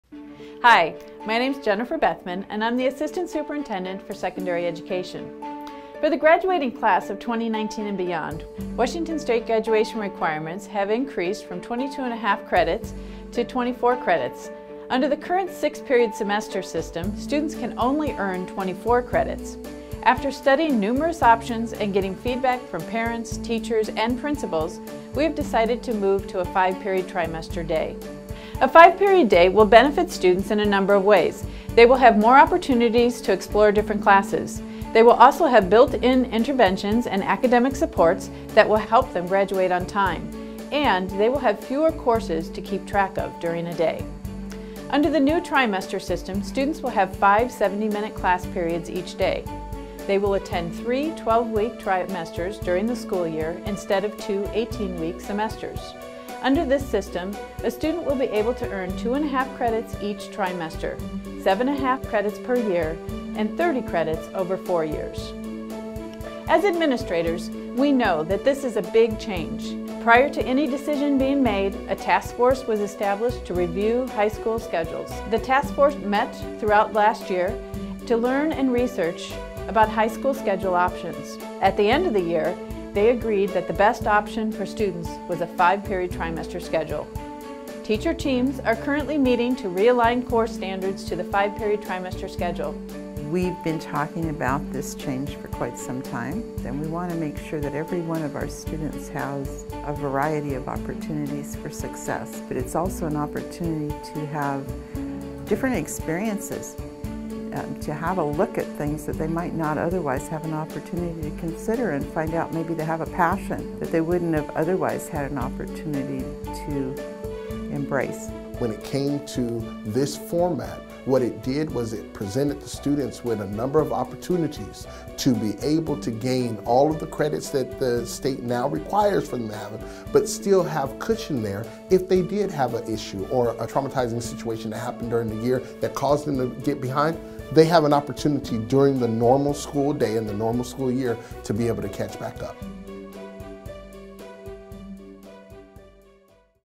Bethel high schools moving to trimester system
For the graduating class of 2019 and beyond, all Bethel high schools will shift from the current six-period semester to a five-period trimester system. The move to trimesters will help meet Washington’s new graduation requirements, which recently increased from 22.5 credits to 24 credits.